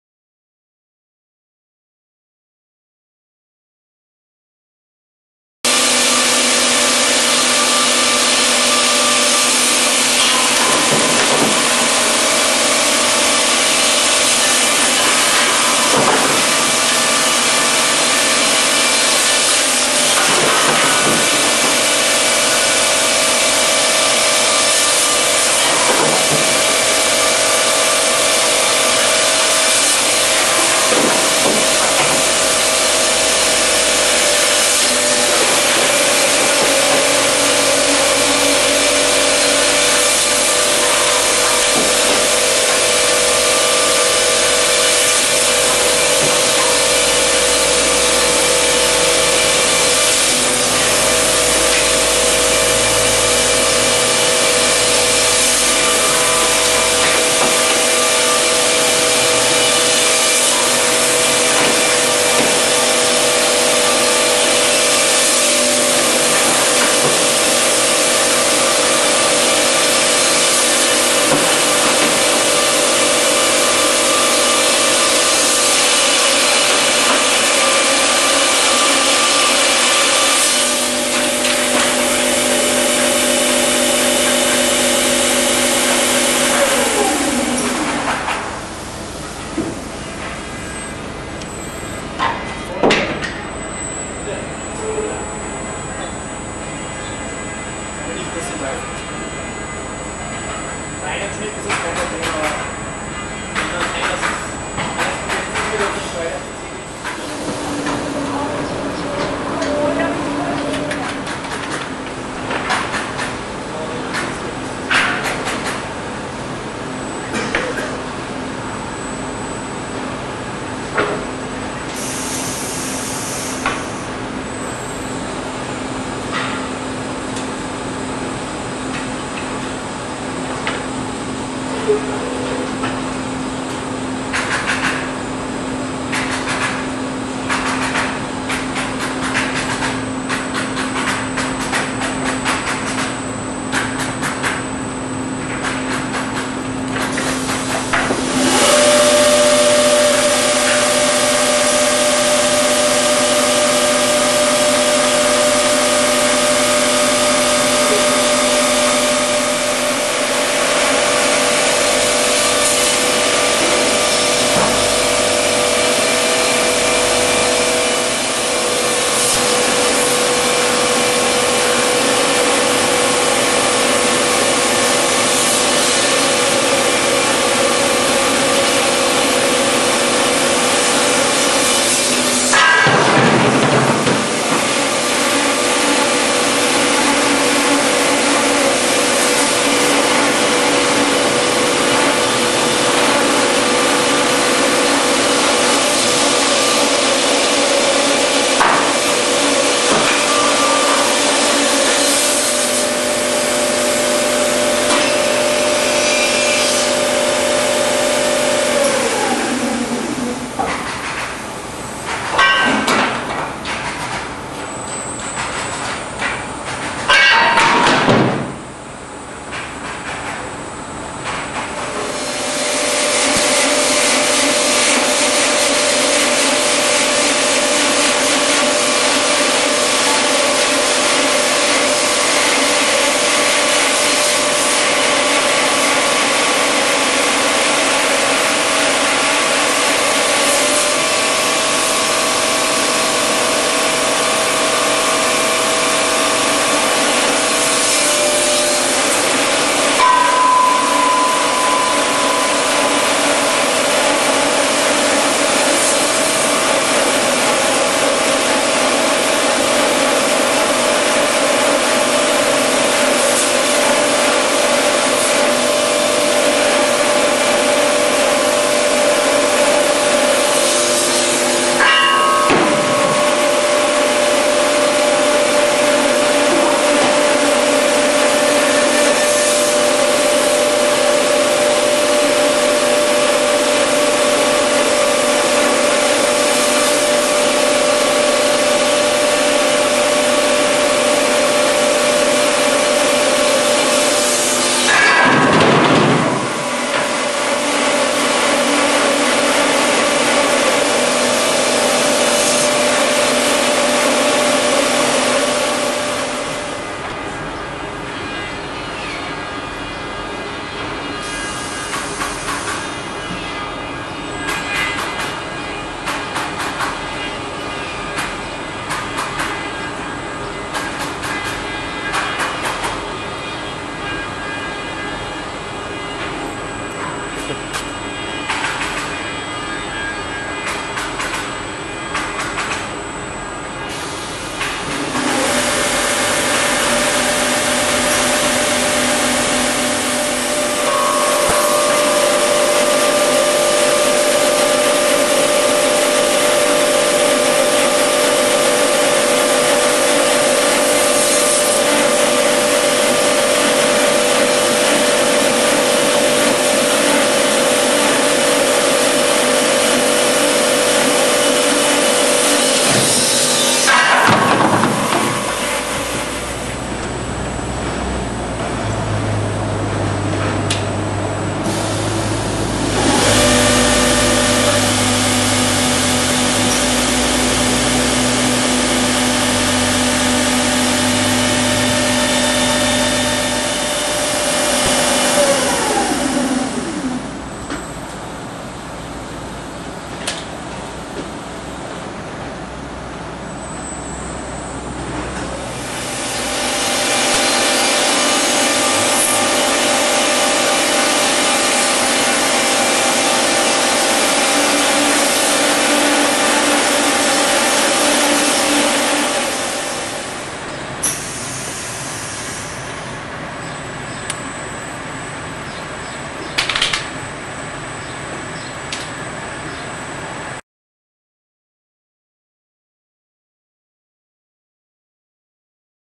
PC120 w pełni automatyczna linia kapowania pakietów #2
Automatyczna linia PC120 do kapowania pakietów tarcicy i płyt, ze stołem rolkowym doprowadzającym i odprowadzającym cięty materiał. Centralnie umieszczona jednostka tnąca. Linia każdorazowo dopasowywana do indywidualnych potrzeb klienta.